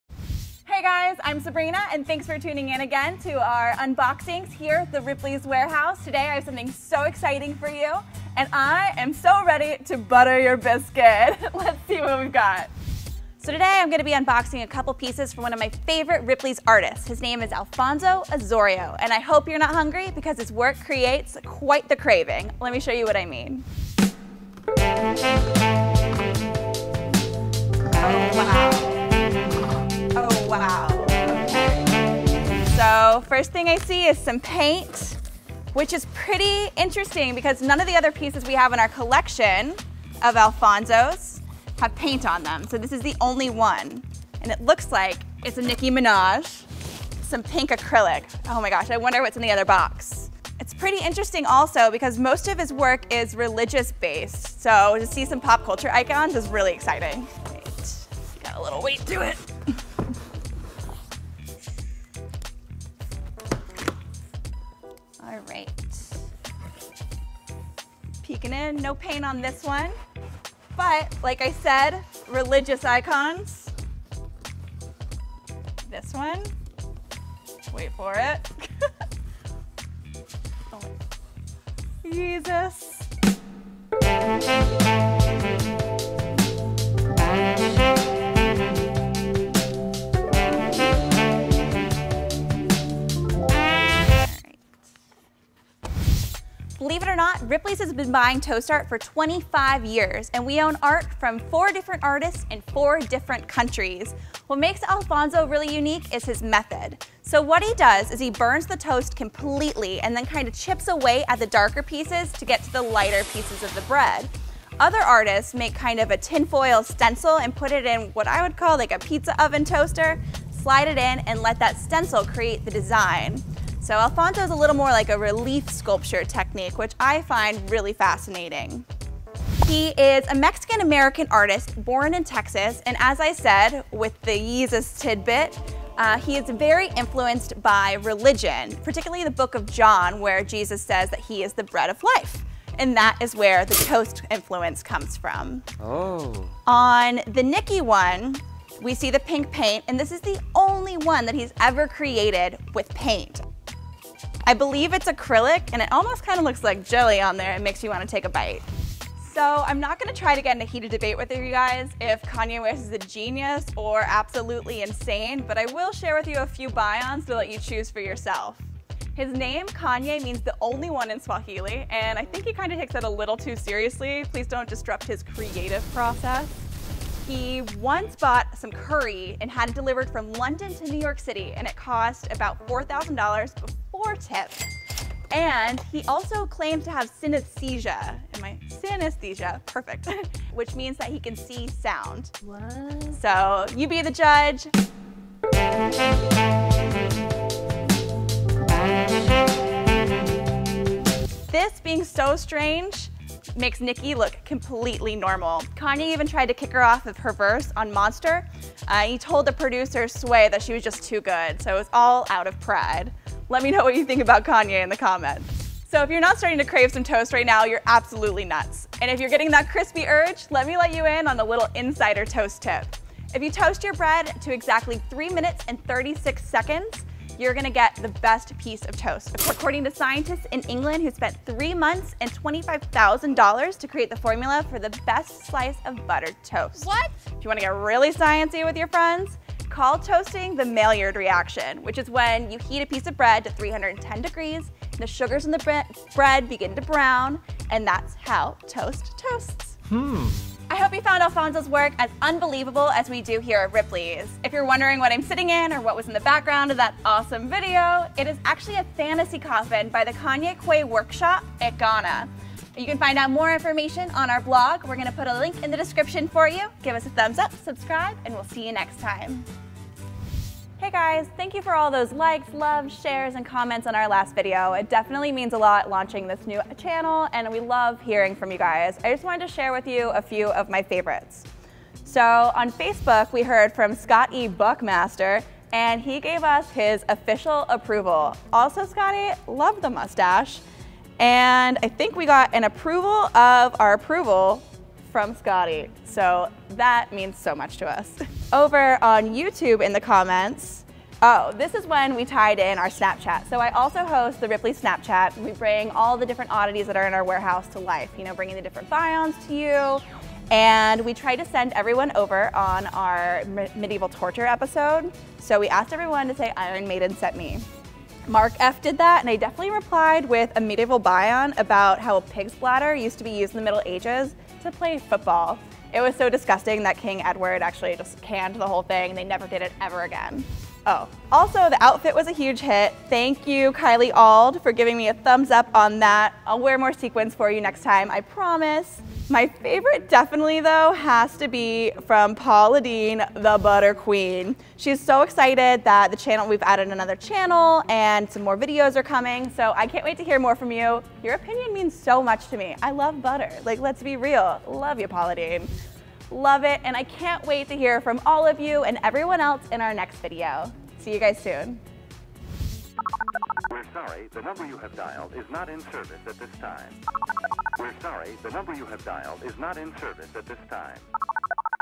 Kanye West & Nicki Minaj Toast Art Unboxing
In this episode, Sabrina unboxes toast art of Kanye West and Nicki Minaj by Texas artist Alfonso Osoria.

Come back next week for an all-new Unboxing Ripley's Believe It or Not! and let us know what you think about the Yeezus toast in the comments.